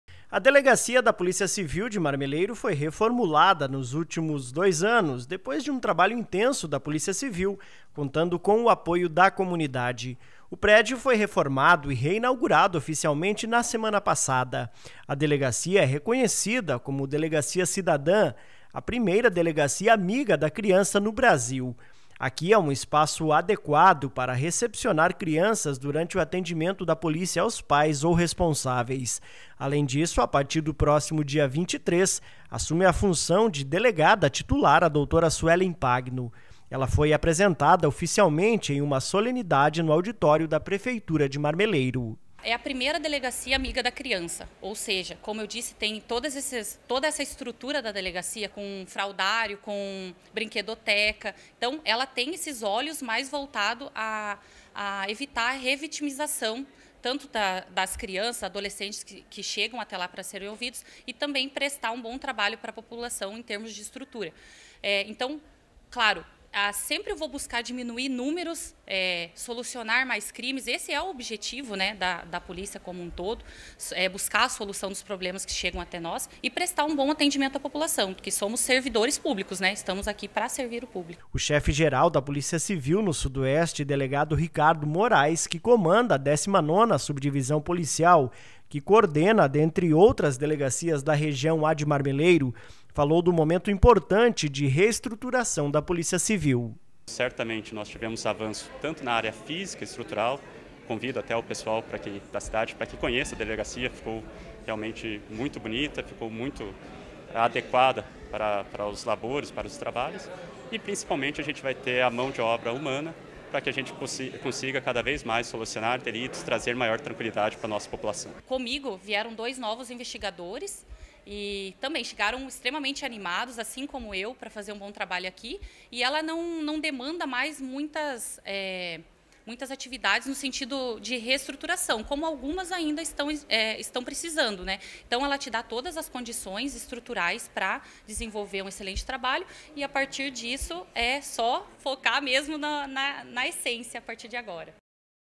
Delegacia de Marmeleiro recebe delegada para chefiar os trabalhos
Segurança: A delgada titular assume o lugar do delegado Fabiano de Oliveira, transferido a Curitiba.